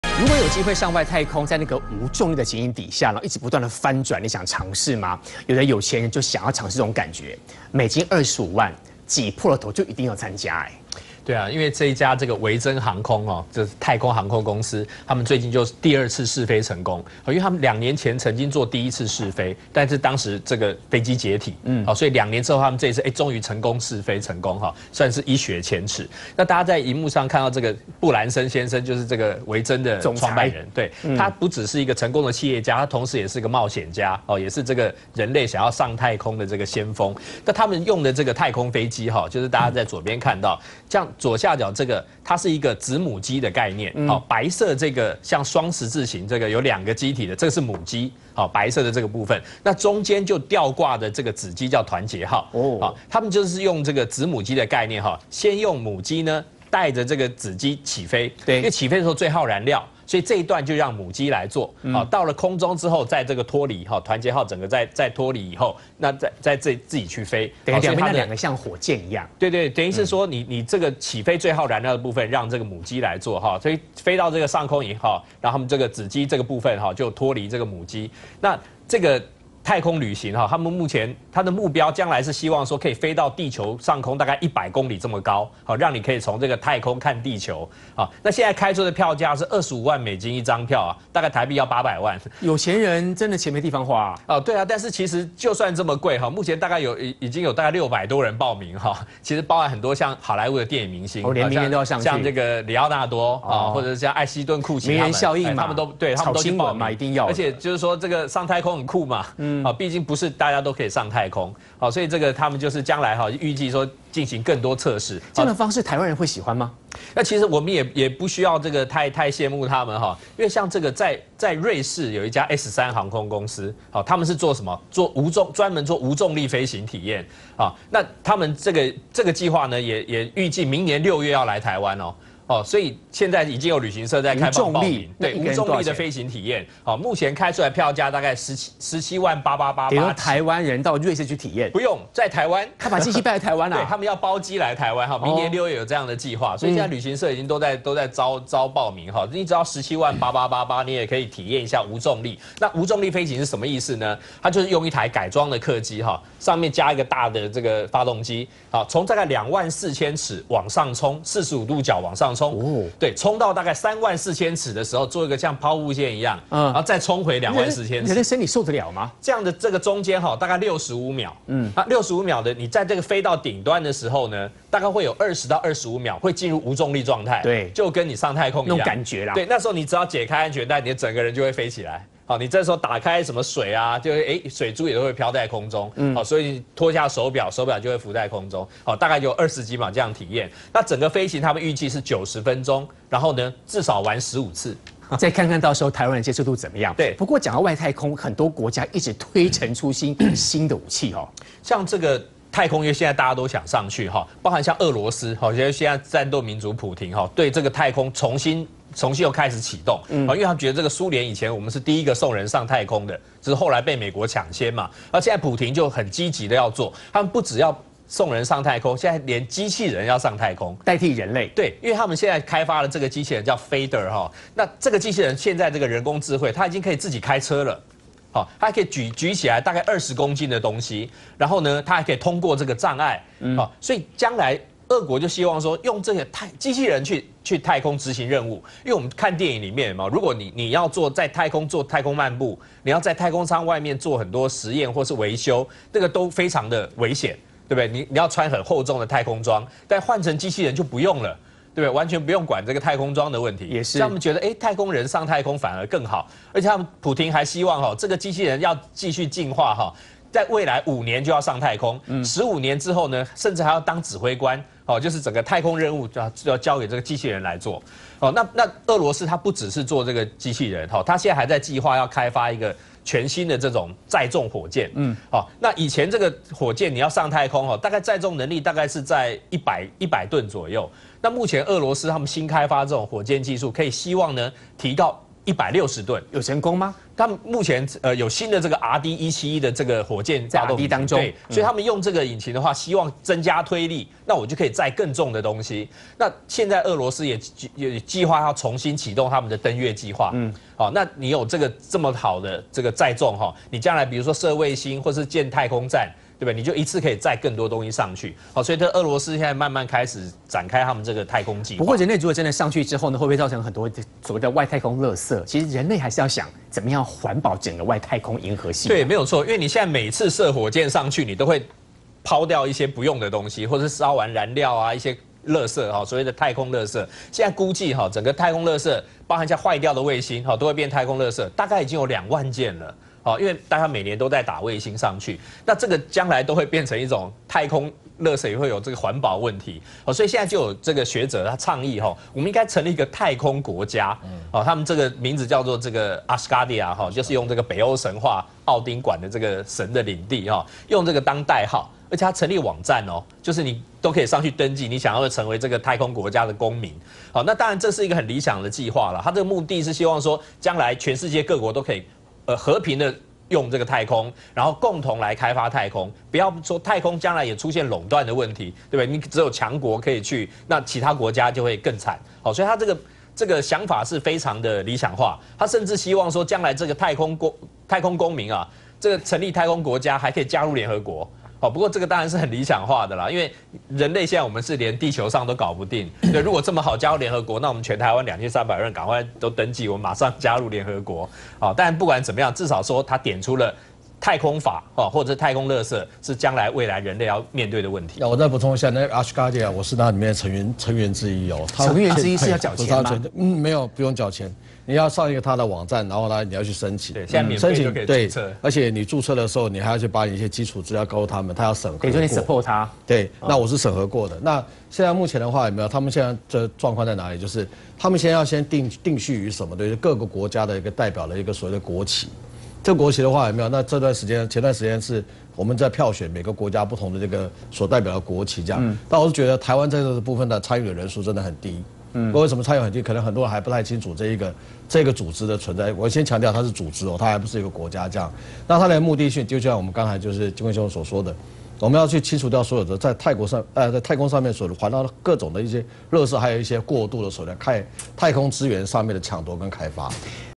挑戰新聞軍事精華版--維珍銀河成功試飛太空船，未來將載名人遊太空
★維珍銀河成功試飛太空船未來將載名人遊太空.(自由時報
★俄羅斯超級機器人　未來上太空執行任務 (蘋果日報
★俄設計全球最大超重型運載火箭:載重160噸 (俄羅斯中文網
★第一個太空國家「Asgardia」招募10萬公民，你報名了嗎？(數位時代